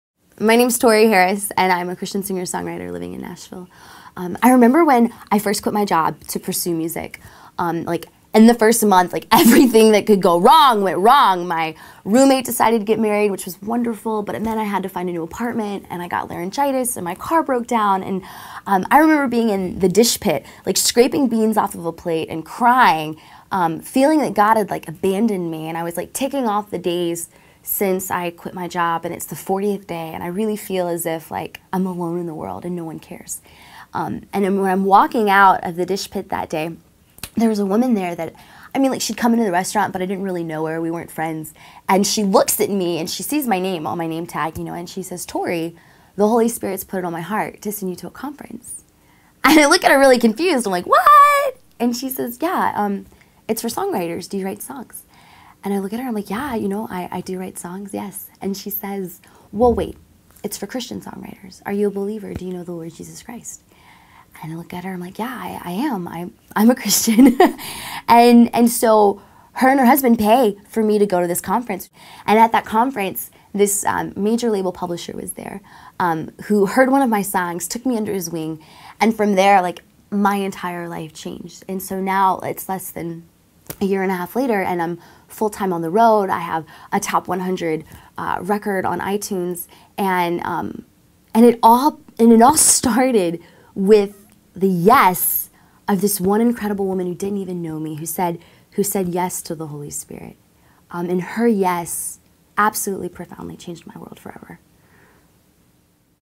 Tori Harris - The Power of One YES - Interview in Studio
Tori Harris tells the story of how she came to become a full-time recording artist and the "yes" of one person who made it possible.